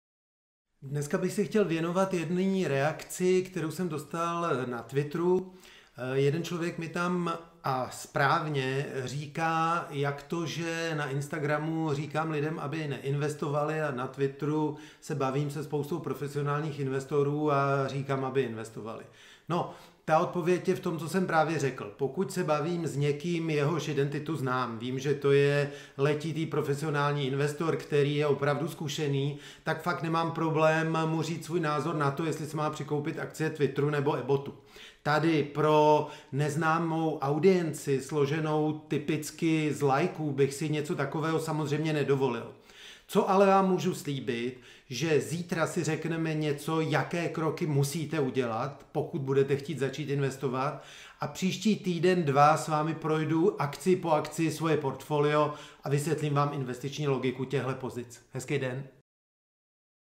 Korona vs. Koruna #27 - NENÍ INVESTOR JAKO INVESTOR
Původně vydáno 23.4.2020
Nedávno se jeden človek na Twitteru podivoval, proč na Instagramu lidi odrazuji od investování, zatímco ve svých tweetech tvrdím pravý opak. Dneska vám v Koroně vs. Koruně vysvětlím, proč tomu tak je. Je to vlastně celkem jednoduché. A od dalších dílů se můžete těšit na konkrétní rady, jak začít s investováním.